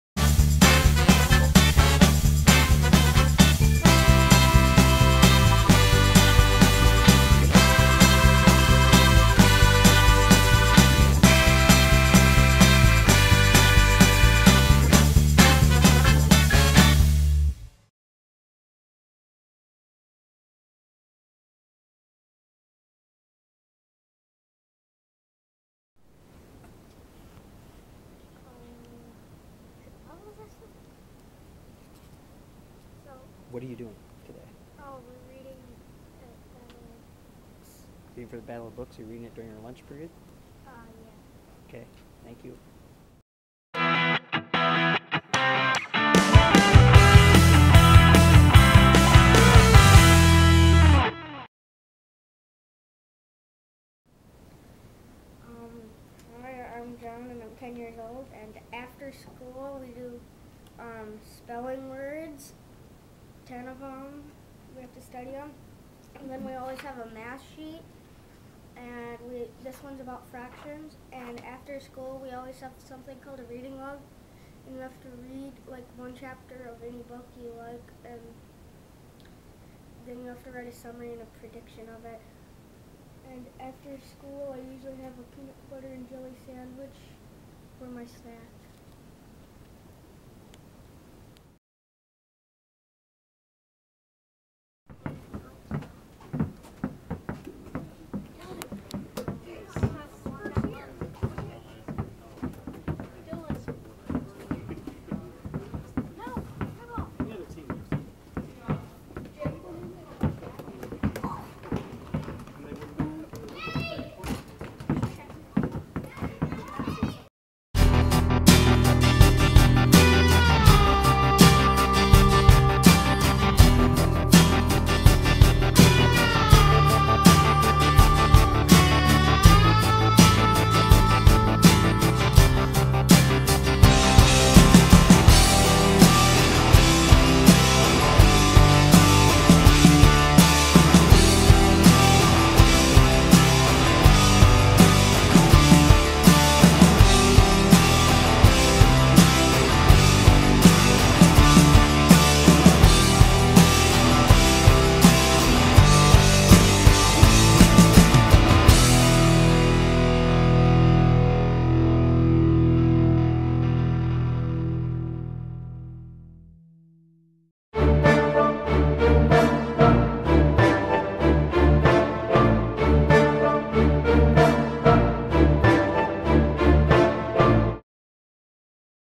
Day in the Life of Joey and John
Another video documenting what life is like for students in Iowa.